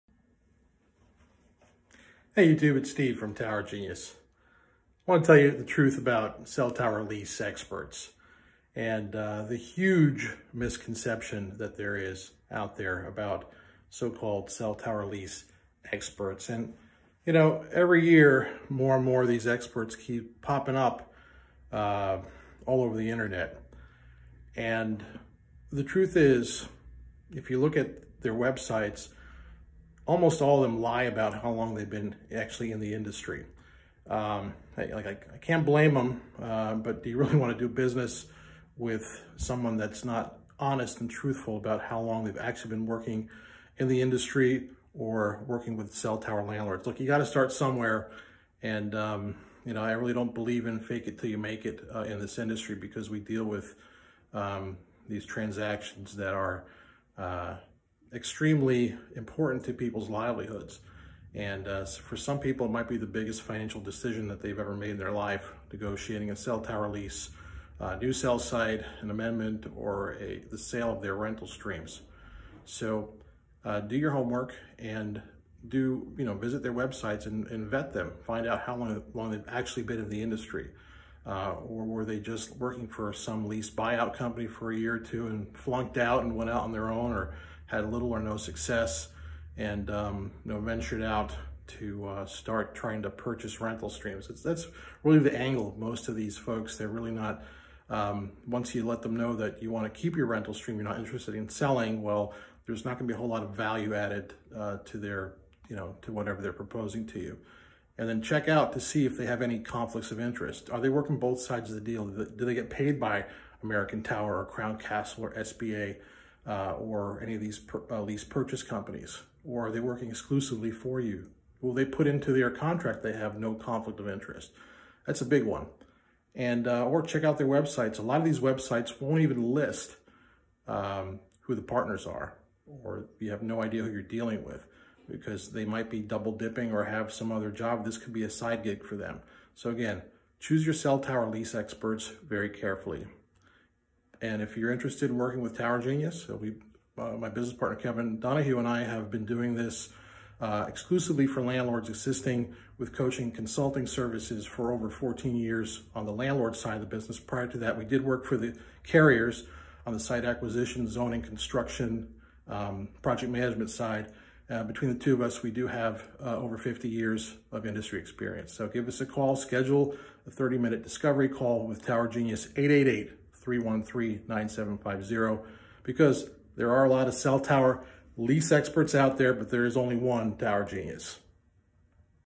How to Evaluate Cell Tower Lease Experts
How to evaluate cell tower lease experts? Do your homework. Looking their backgrounds. Most of they are only focused on "lease buyouts" and add little or no value if you are not interested in selling your rental stream.
What about their actual years of experience in cell tower industry? Most landlord consultants are not truthful about how long they have in the cell tower industry. Do you really want to do business with companies who lie about how long they have been doing this? Many cell tower lease advisors have conflicts of interest. That is they get paid by you and they also get paid on the back end. It happens. We've been offered compensation by major tower companies to help "grease the wheels" of a negotiation client.